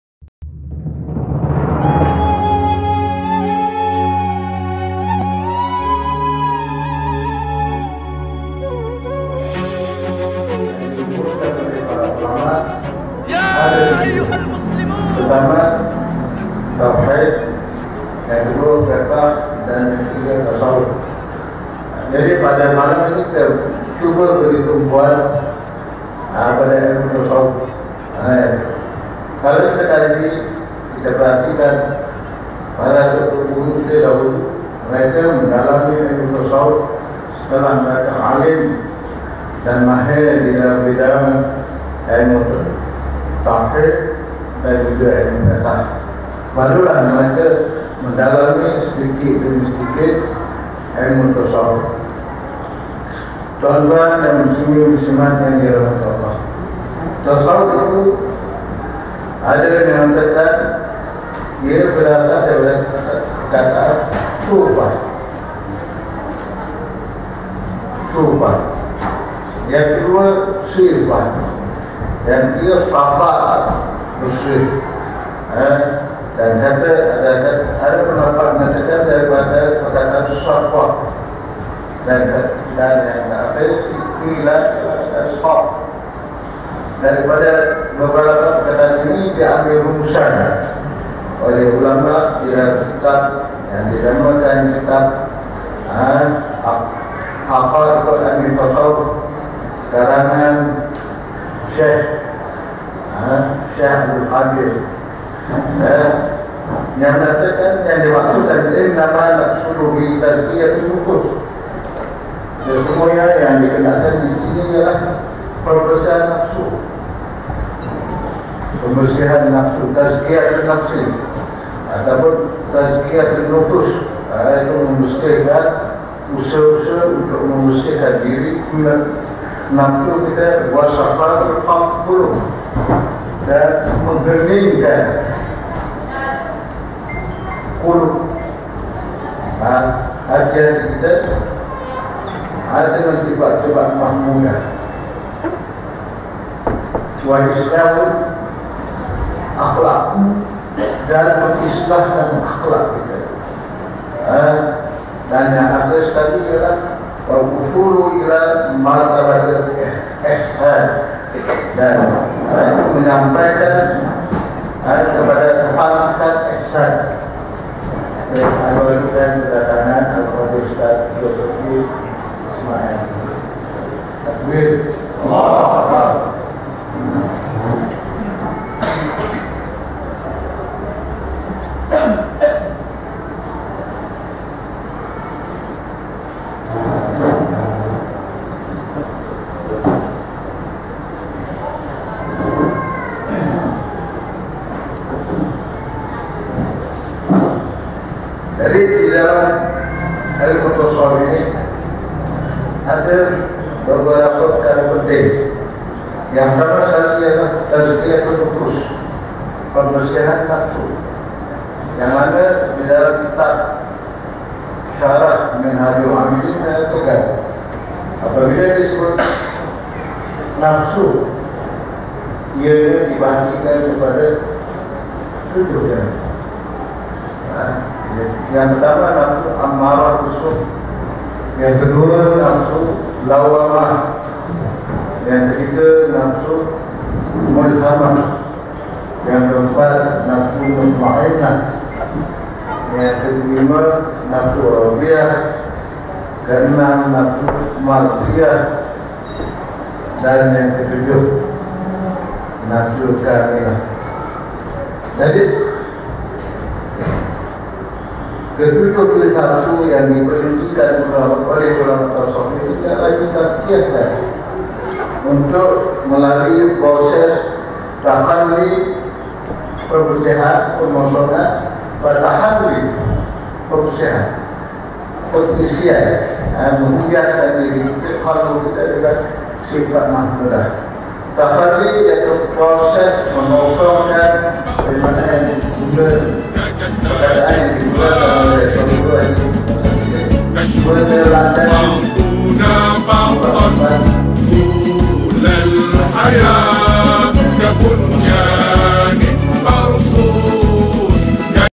Ustaz Sabri Hj Sabran - Pembahagian Nafsu - Bahagian 3
Seminar Kesufian 3.0 anjuran Dewan Ulamak PAS Perak pada Sabtu jam 9.00 malam 14 November 2015 bertempat di Markaz Tarbiyah Negeri, Air Kuning, Taiping, Perak.